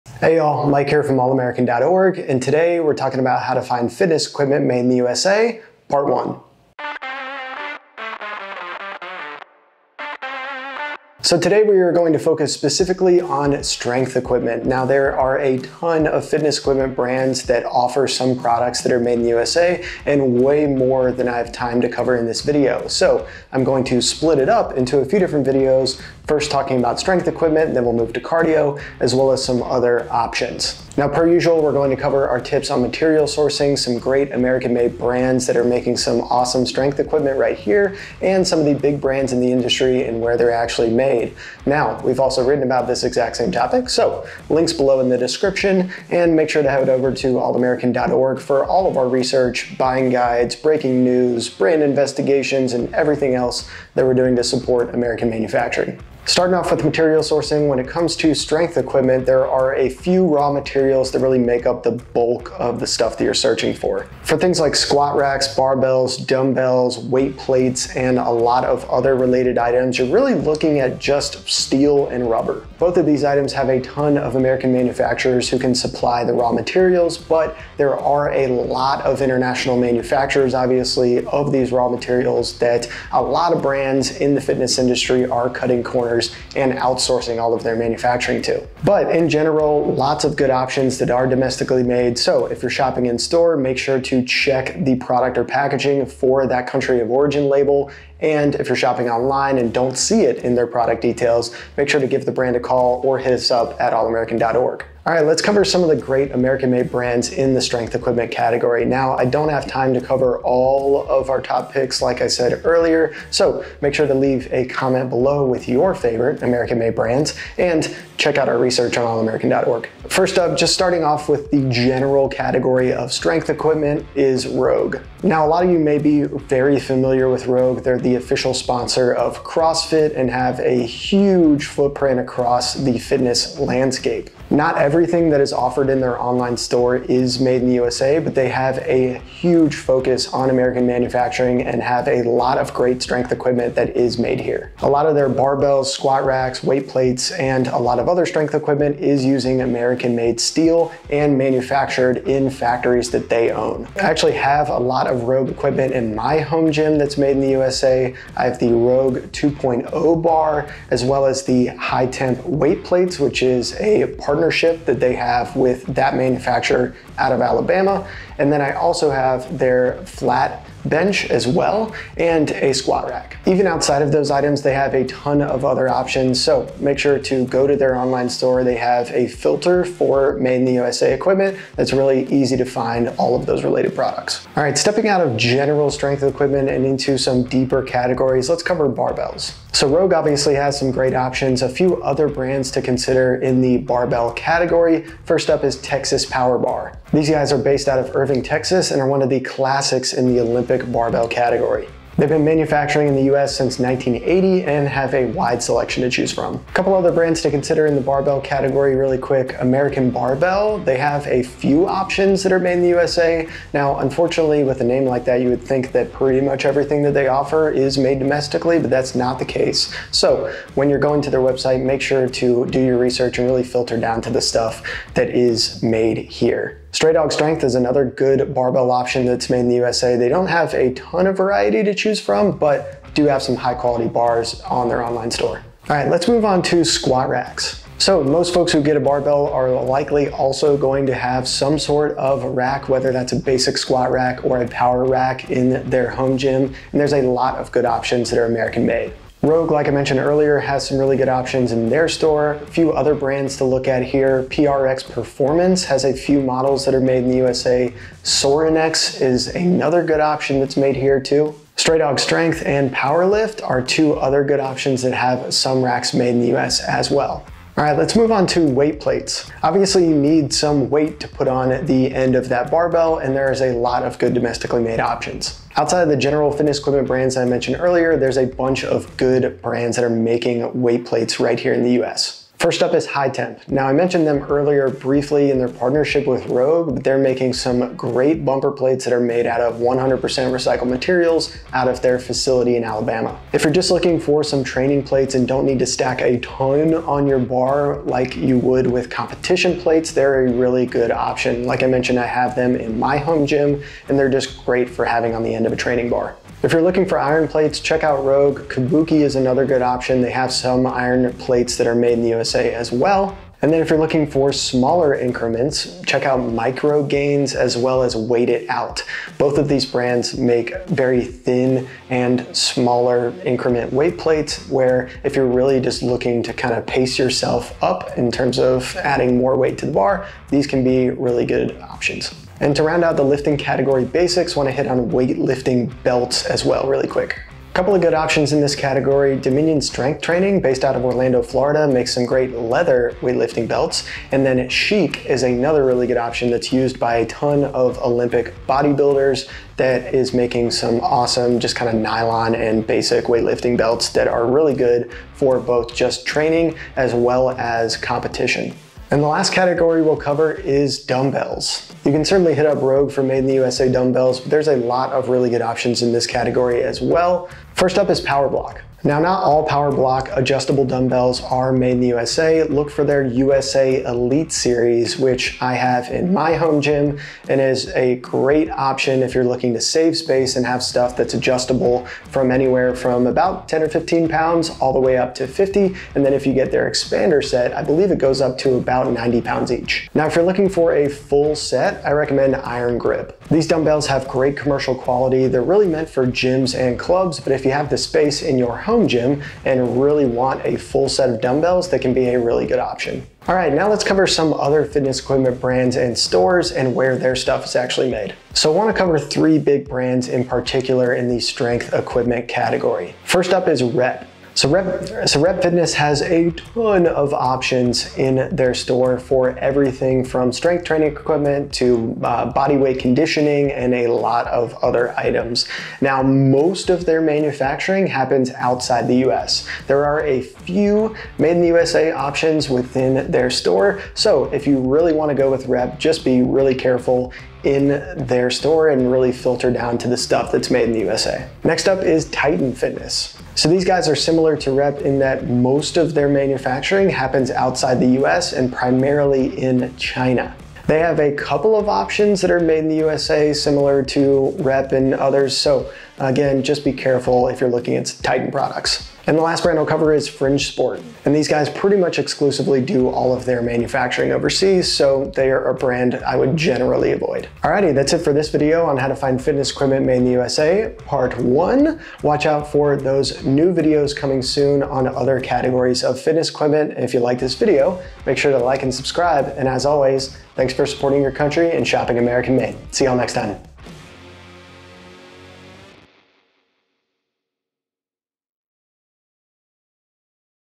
How to Find Weightlifting Equipment Made in the USA For Your Home Gym (+ 15 American Made Brands!)
In this video, we're talking about strength training equipment made in the USA. Home gyms take a lot of work to build, and you want to put a ton of care into the space and fitness equipment you're putting in it - it's a sacred place after all! There are a lot of options out there for strength training equipment, a bunch of which is manufactured overseas and not going to last you a long time. So, we cover how to find the great American made strength training brands, highlight some great companies, and cover some "watch outs" for brands that aren't made in the USA. We cover everything from barbells to squat racks, weight plates, benches, dumbbells, and more, so strap in.

Sections:
0:00 Intro
0:57 How to Find Strength Training Equipment Made in the USA
1:45 American Made Strength Training Equipment Brands
6:43 Where Are The Big Brands Made?